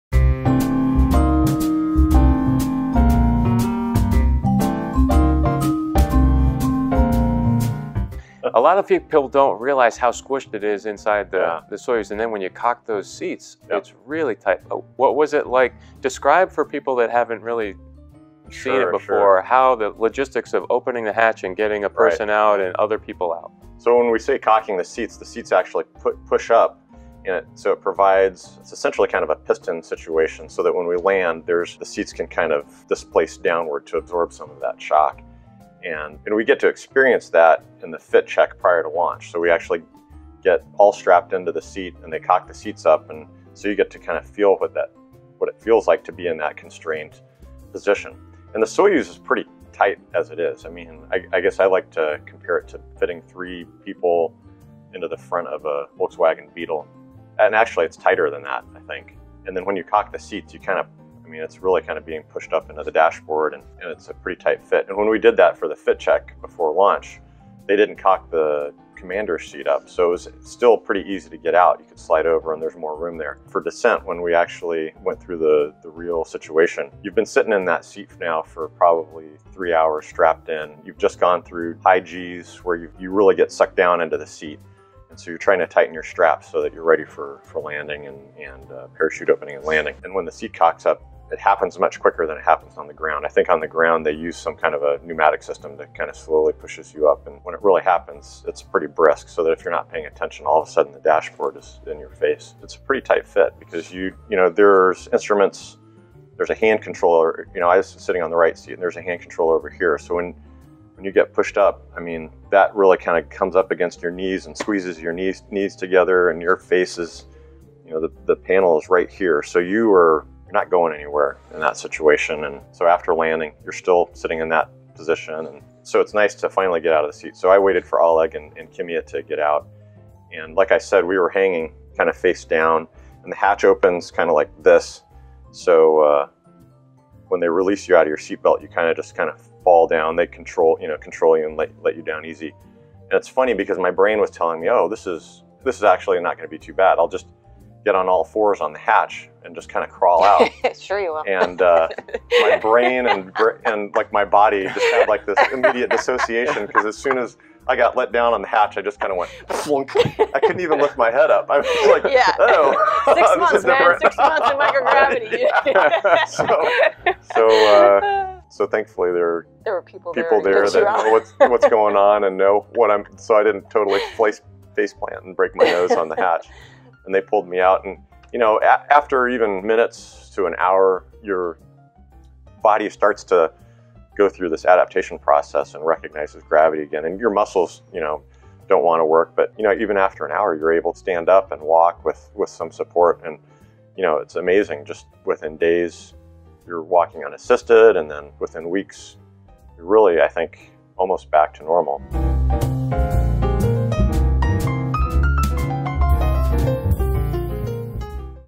Cramped Ride From Orbit Via Soyuz | Video
When Soyuz capsule seats ‘cock,’ it’s tighter than ‘fitting three people into the front of a Volkswagen Beetle” says NASA Astronaut Kjell Lindgren. With astronauts Chris Cassidy and Tracy Dyson, Lindgren reminisces about this squashy right of passage back to the home-world. -- Soyuz Snaps Amazing View of Space Station Dock Switch | Time-Lapse Video

Credit: NASA